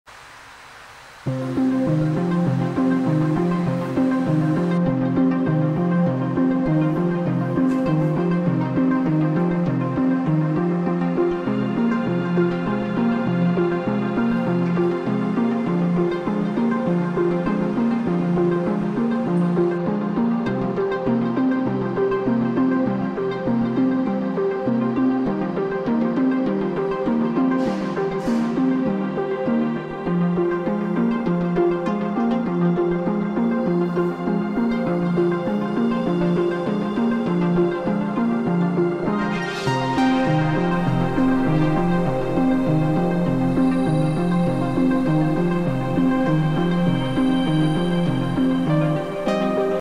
Hayfa'daki Rambam Hastanesi otoparkının büyük bir bölümü acil servise dönüştürüldü
İsrail'in kuzeyinde bulunan Hayfa kentindeki Rambam Hastanesi otoparkının büyük bir bölümü acil servise dönüştürüldü.